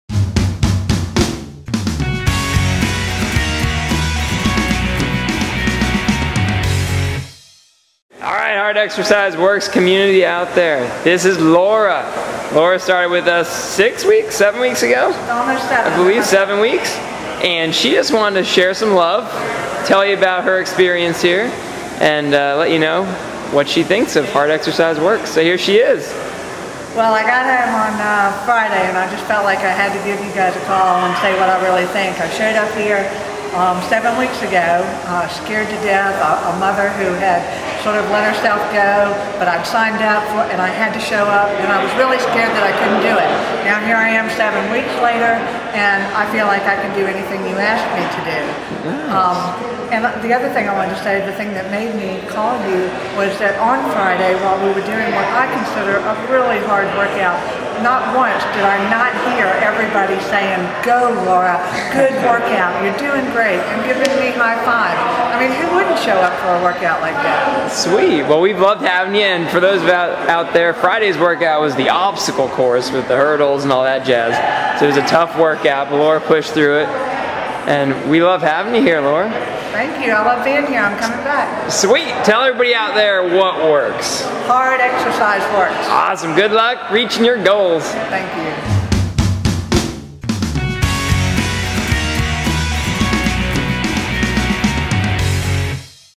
Laura's Testimonial on Why "HEW" is Awesome.m4v
Great testimonial from Laura here at Hard Exercise Works Asheville. Thanks for your dedication Laura - you are doing great and we love having you in our crazy community :-). Keep it up!